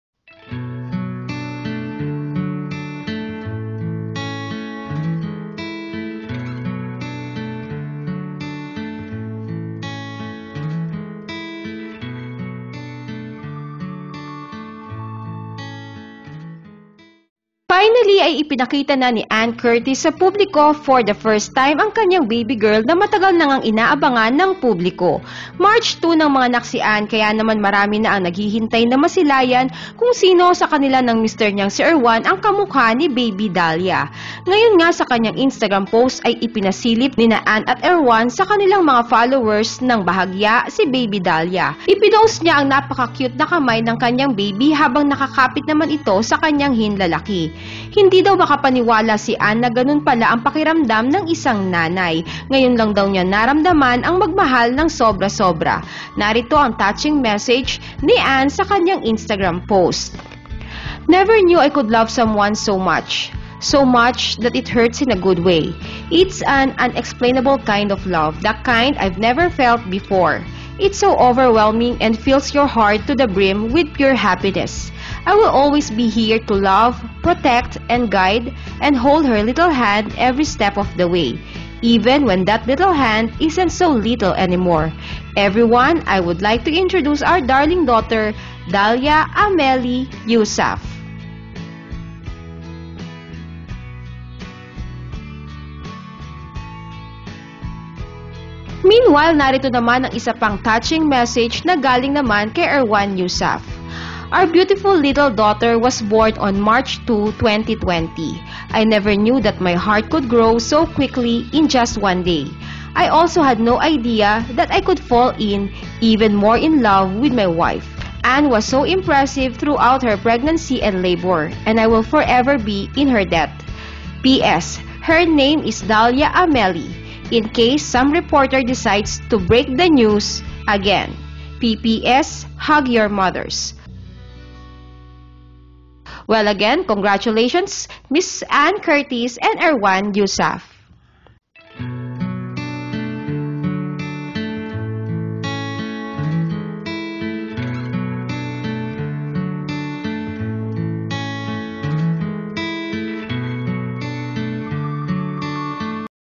Baby ni ANNE CURTIS ang CUTE Ipinakita na sa PUBLIKO!!
Finally ay ipinakita na ni Anne Curtis sa publiko for the first time ang kanyang baby girl na matagal nangang inaabangan ng publiko.